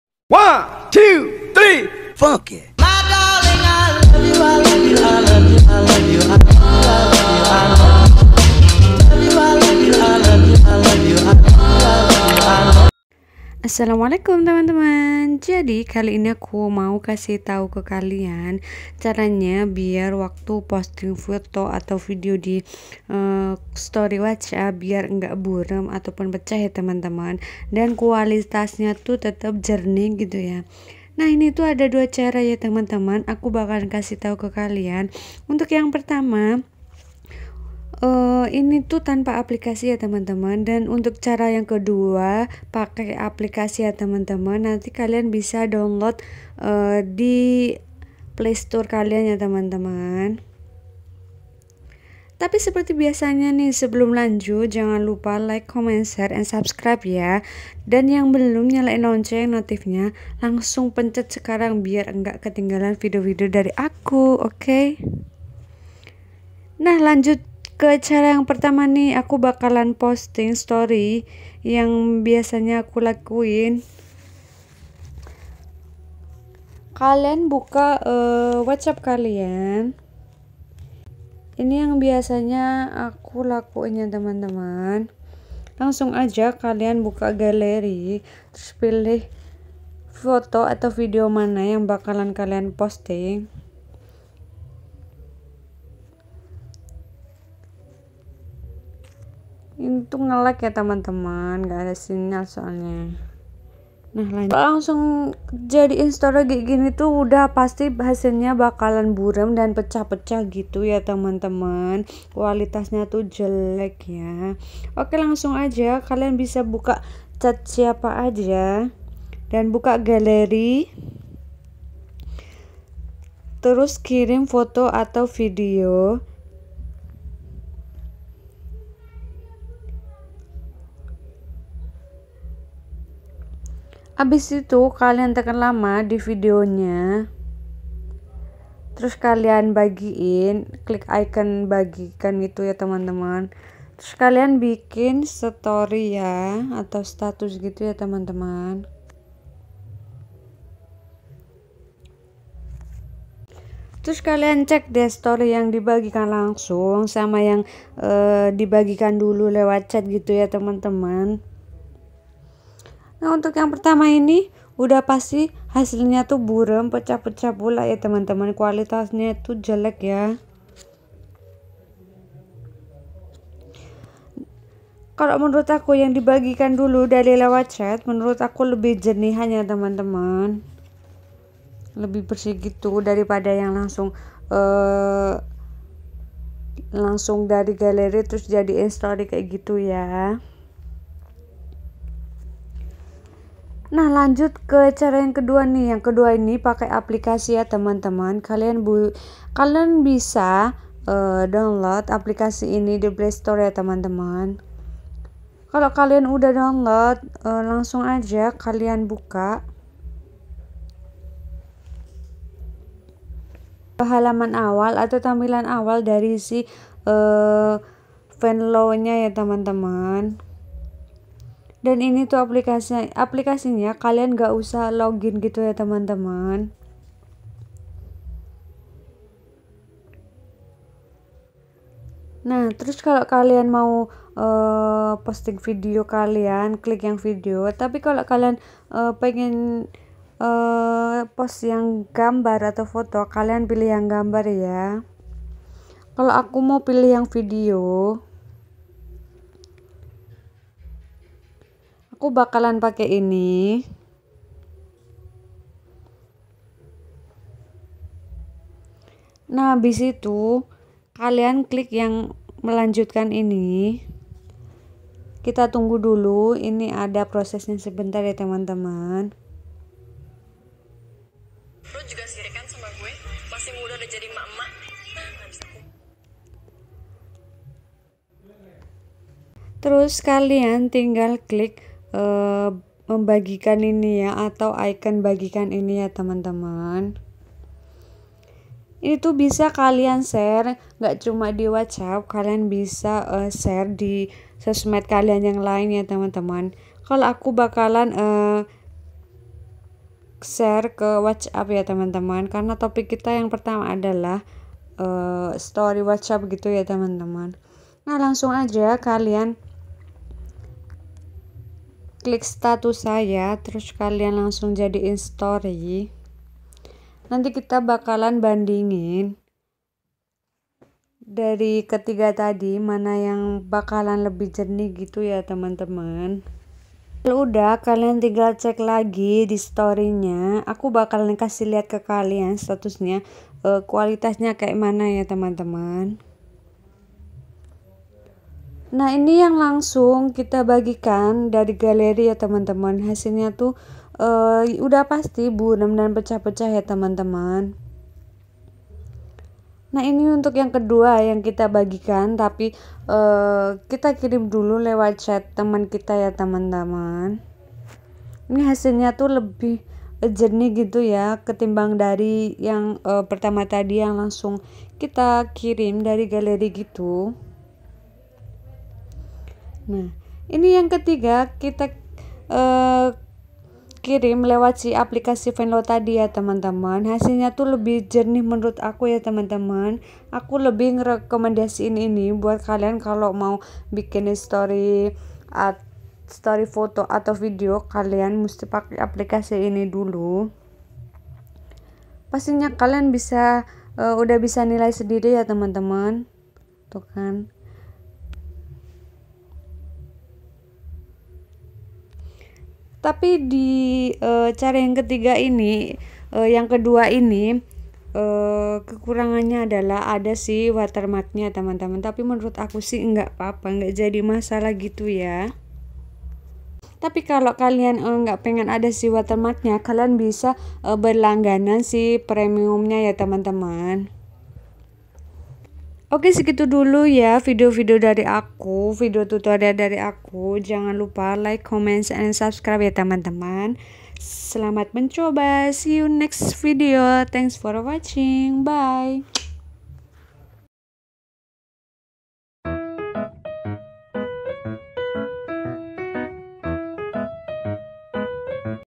CARA AGAR UPLOAD STORY WHATSAPP TIDAK BURAM / PECAH ll
hallo guys .
sebelumnya thanks udah nonton video aku ..☺️
oke di video kaali ini aku bakalan kasih tutorial ke kalian gimana sih cara agar waktu upload si foto / video di whatsapp biar engga burik ...dulu itu padahal jernih yaa temen" ...tapi gatau kenapa si Whatsapp sekarang kok kalo upload kualitasnyanya tuh burik kan yaa ... Lengkapnya kalian bisa tonton video aku ini ..jangan skip" biar engga gagal faham yaa temen" .. happy watching 🍀

📍device 📍
vivo y11
tripod 26cm
inshot